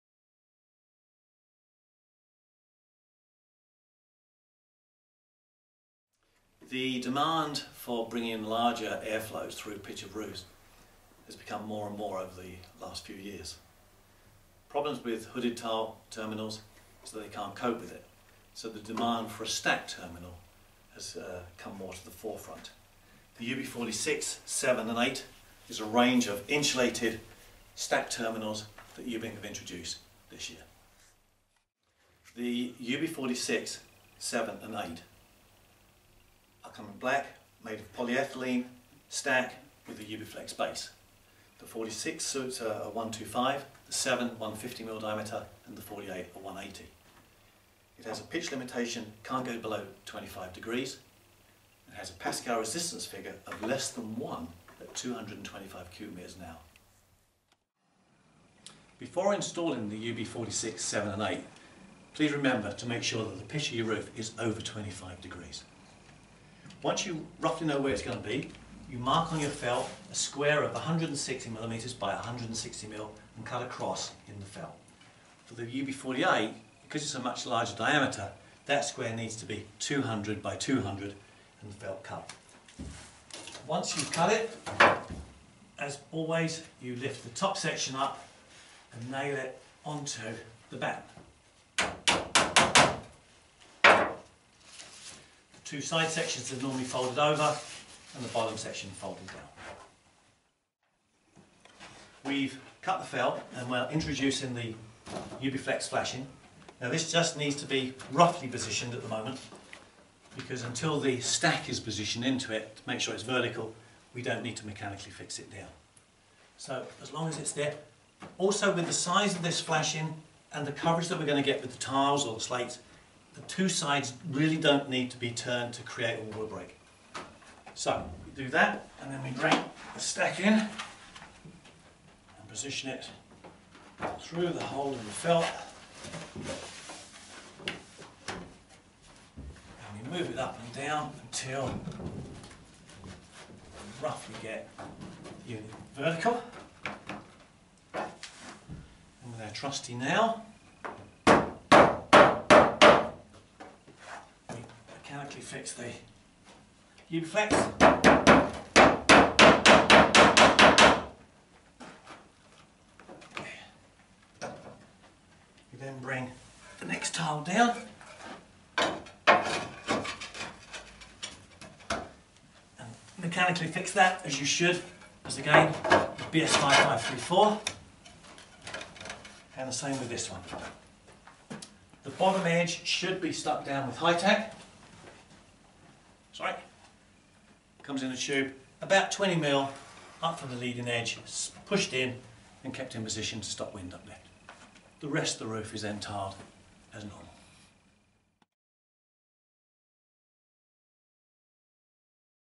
Ubbink UB46 47 48 insulated stacked terminal
Ubbink universal insulated terminals are suitable for both tiled and slated roofs and come complete with a Ubiflex flashing. These units are suitable for roof pitches over 25° and for mechanical extraction.
ption